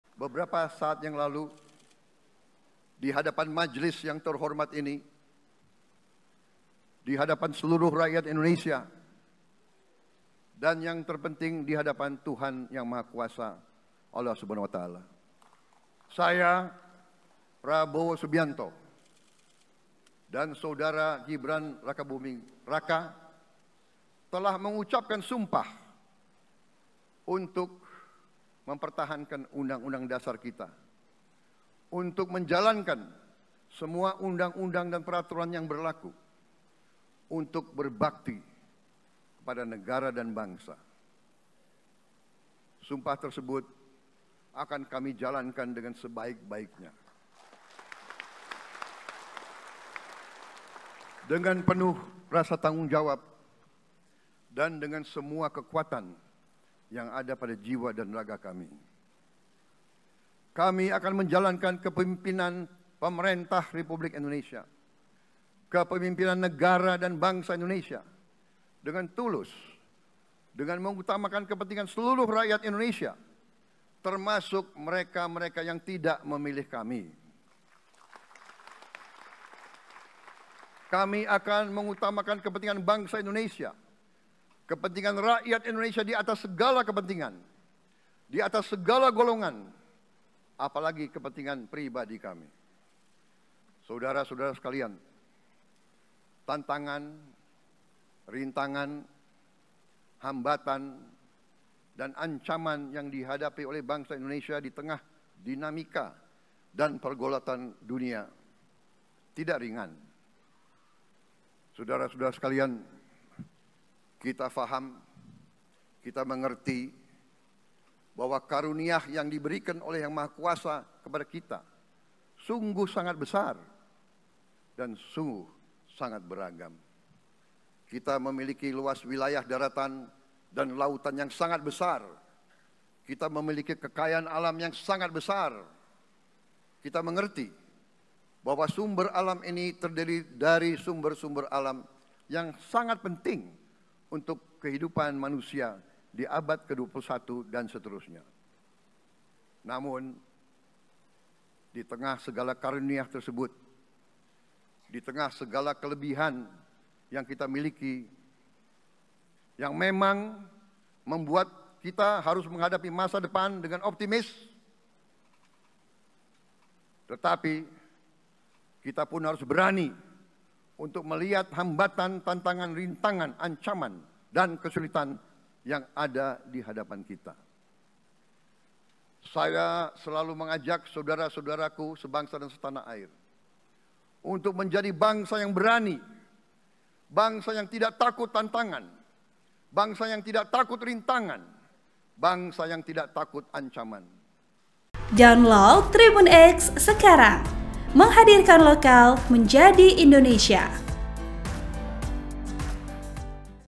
Prabowo Resmi Jadi Presiden RI 2024-2029, Berjanji Utamakan Kepentingan Rakyat
Prabowo Subianto resmi menjadi Presiden Republik Indonesia, Minggu (20/10/2024).
Lewat pidato perdananya sebagai presiden, Prabowo berjanji akan mengutamakan kepentingan bangsa Indonesia dan menyebut kepentingan rakyat Indonesia di atas segala kepentingan.

Editor Video : Mohamad Afkar Sarvika